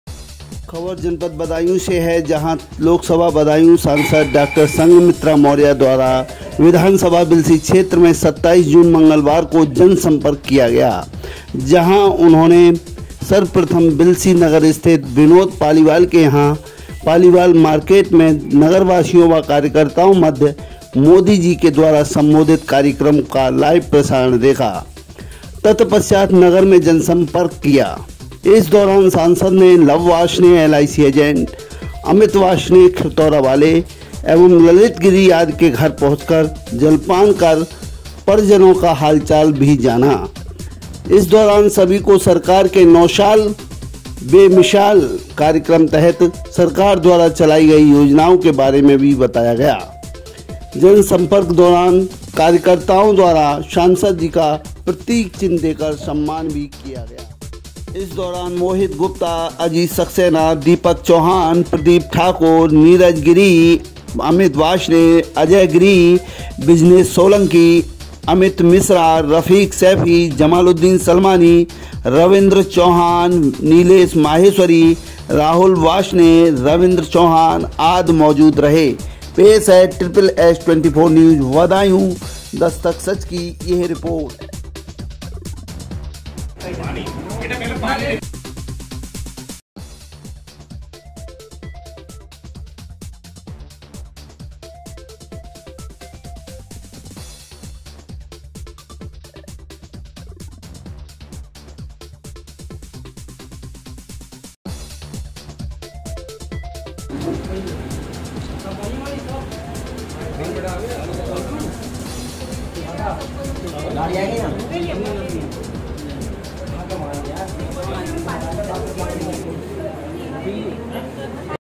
Badaun : बदायूॅं सांसद ने किया जनसंपर्क, प्रतीक चिन्ह देकर किया सम्मान
लोकसभा बदायूॅं सांसद डॉ० संघमित्रा मौर्या ने मंगलवार को विधानसभा बिल्सी क्षेत्र में सरकार के 09 साल बेमिसाल कार्यक्रम तहत जनसंपर्क किया.